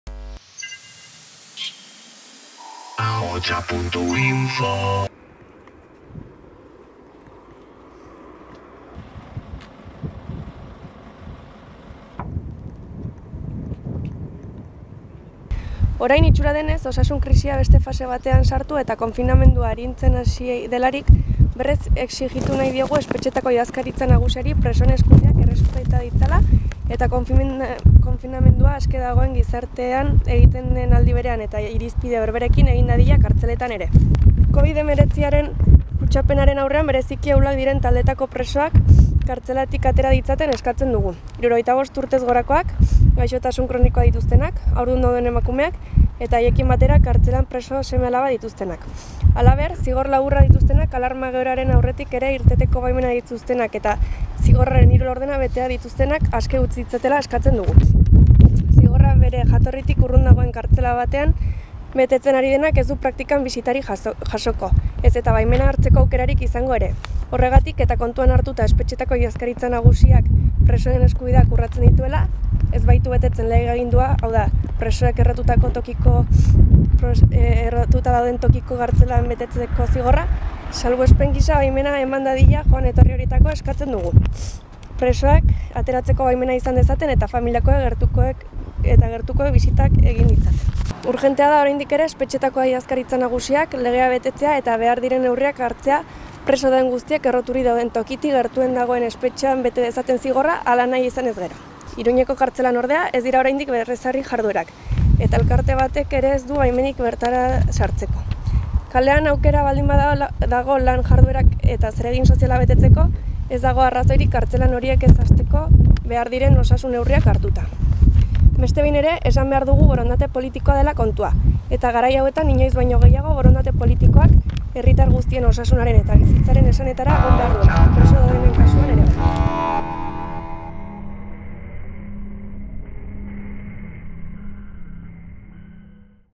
Presoen eskubideak errespetatzea eskatzen diote Espainiako Gobernuari
Babes politiko eta sindikal oso garrantzitsua eta zabala jaso du Etxerat, Sare, Salhaketa eta Altsasu Gurasoak bultzatzen duten SOS Presoak Covi19 manifestuak. Nafarroako sindikatu guztiek egin dute bat eskariekin: ELA, CCOO, UGT, LAB, STEILAS, ESK, HIRU, CNT, CGT-LKN, SOLIDARI eta EHNE. Geroa Bai, Eh Bildu, Podemo-Ahal Dugu eta Izquierda-Ezkerra ere batu dira presoen eskubideen aldeko aldarriei.

"SOS Presoak Covid19" manifestuak babes anitza eta zabala jaso du. Salhaketa Nafarroak, Etxeratek, Altsasuko Gurasoak plataformak eta Sare Herritarrak elkar hartuta abian jarri duten kanpainaren emiatzak gaur aurkeztu dituzte. Alderdi eta sindikatu gehienen babesaz aparte, hainbat elkarte eta kolektiboen atxikimendua ere jaso dute. Manifestu honekin dei egin diote Espainiako Gobernuari presoen eskubideak errespetatzen dituzten neurriak har ditzala COVID-19ari aurre egiteko.

"Orain, itxura denez osasun krisia beste fase batean sartu, eta konfinamendua arintzen hasia delarik, berriz exijitu nahi diogu Espetxeetako Idazkaritza Nagusiari presoen eskubideak errespeta ditzala eta deskonfinamendua aske dagoen gizartean egiten den aldi berean eta irizpide berberekin egin dadila kartzeletan ere", esan dute.

Espainiako Gobernuari eskatzen diote Covid19aren kutsapenaren aurrean bereziki ahulak diren taldeetako presoak berehala askatzea: 65 urtez gorakoak, gaixotasun kronikoak dituztenak eta haurdun dauden emakumeak edo haiekin batera kartzelan preso seme-alabak dituztenak. Eskatzen dute, halaber, aske utz ditzatela zigor laburrak dituztenak, alarma egoeraren aurretik ere irteteko baimenak zituztenak eta zigorraren hiru laurdenak beteak dituztenak.

Egia bada ere berriz hasi direla familiako eta gertukoekiko komunikazioak, kristala tarteko, bai eta espetxeetako baimenetako irteerak ere, bi kasuetan baldintzatuta daude, mugikortasunerako ezarritako mugak bete behar baitira nahitaez (probintziatik ez ateratzea). Horren ondorioz, zigorra bere jatorritik urrun dagoen kartzela batean betetzen ari denak ez du praktikan bisitarik jasoko, ez eta baimena hartzeko aukerarik izango ere. Horregatik eskatzen dute salbuespen gisa baimena eman dadila joan-etorri horietarako, presoek ateratzeko baimena izan dezaten eta familiakoek eta gertukoek bisitak egin ditzaten.

"Urgentea da oraindik ere Espetxeetako Idazkaritza Nagusiak legea betetzea eta behar diren neurriak hartzea preso dauden guztiek erroturik dauden tokitik gertuen dagoen espetxean bete dezaten zigorra", esan dute sakabanaketa politikaren amaiera eskatuz.

Deseskalatze planean jasotzen da, orobat, kartzela barruko jarduerak hasi behar direla berriz, eta hala da joan zen ostegunaz geroztik. Iruñeko kartzelan, ordea, ez dira oraindik berrezarri jarduerak, eta elkarte batek ere ez du baimenik bertara sartzeko. "Kalean aukera baldin badago lan jarduerak eta zeregin sozialak betetzeko, ez dago arrazoirik kartzelan horiekin ez hasteko behar diren osasun neurriak hartuta", esan dute.

Borondate politiko kontua dela diote manifestuaren sinatzaileek, eta gogoratu dute garai hauetan "inoiz baino gehiago, borondate politikoak herritar guztien osasunaren eta bizitzaren esanetara egon behar" duela, baita presoen kasuetan.

Patxi Ruiz presoekin kezkatuak

Patxi Ruiz presoaren egoera ere kontutan hartu dute Salhaketa, Sare, Etxerat eta Altsasu Gurasoak. "Kezka handiz bizi dugu Patxi Ruiz presoaren egoera. Salbuespenezko espetxe politikaren ondorioa" dela salatu dute. "Holako gertaerak ez gertatzeko mendekuan oinarritutako espetxe  politikarekin amaitu behar" dela aldarrikatu dute.